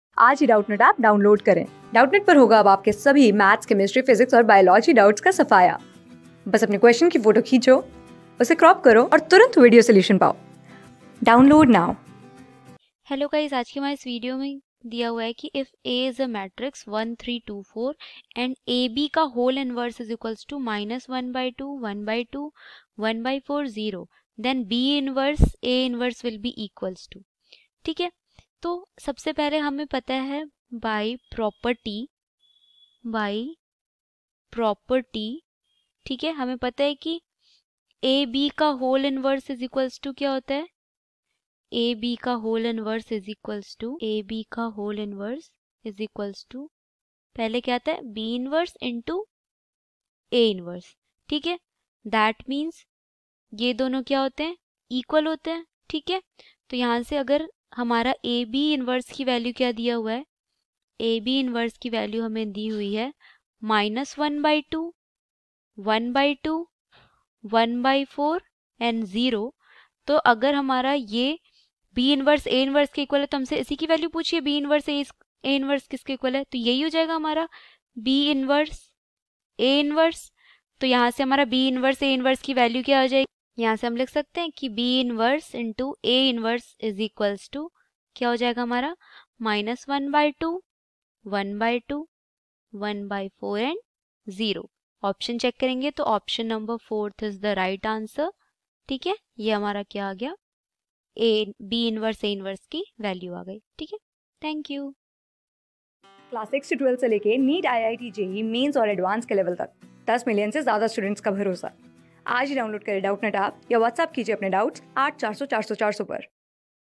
If A=[(1,3),(2,4)] and (AB)^(-1)=[((-1)/(2),(1)/(2)),((1)/(4),0)], then B^(-1). A^(-1)= | CLASS ...
If A=[(1,3),(2,4)] and (AB)^(-1)=[((-1)/(2),(1)/(2)),((1)/(4),0)], then B^(-1). A^(-1)=
Class: 12
Subject: MATHS
Chapter: MATRICES
Board:IIT JEE

👉 Have Any Query? Ask Us.